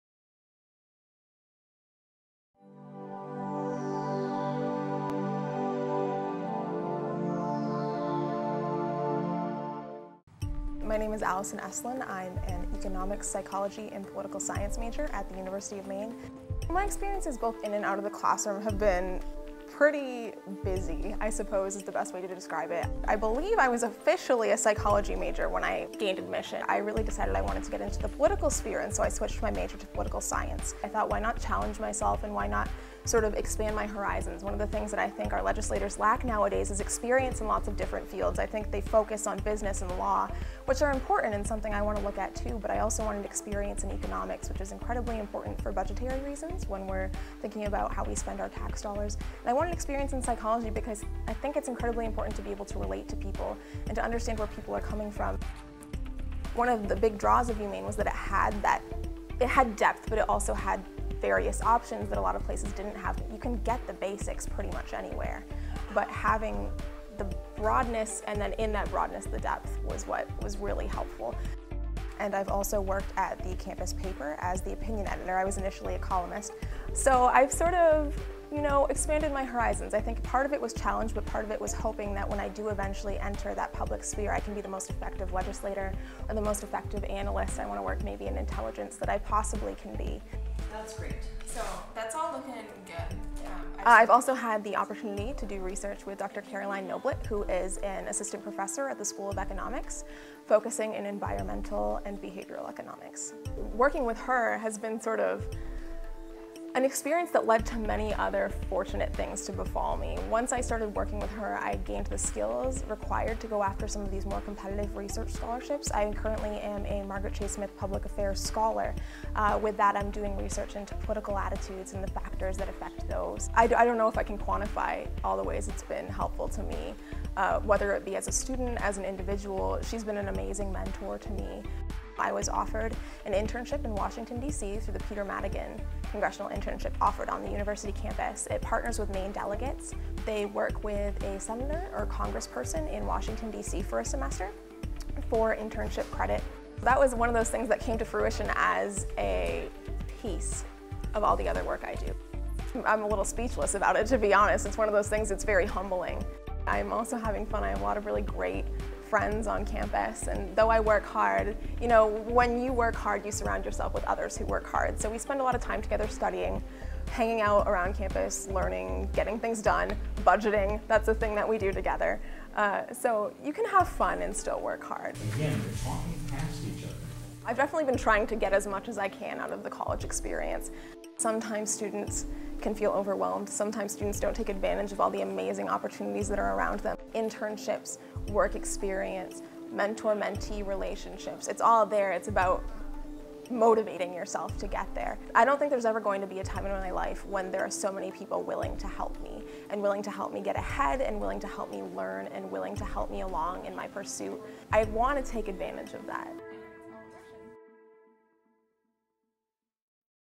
The life of a triple major
Allyson Eslin, a triple major in political science, psychology and economics talks about combining her academic experience with her work as a research assistant, editor for the campus newspaper and congressional intern